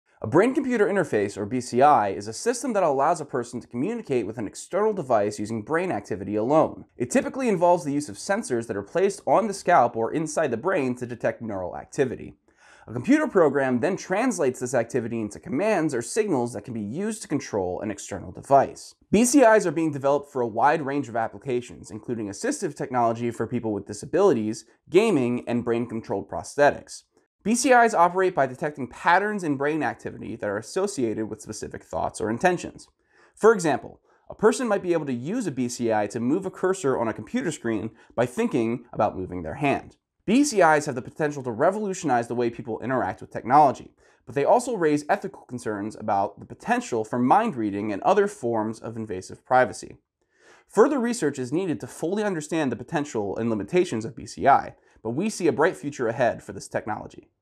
What is a Brain-Computer Interface?
Here's what a brain-computer interface is and how it works!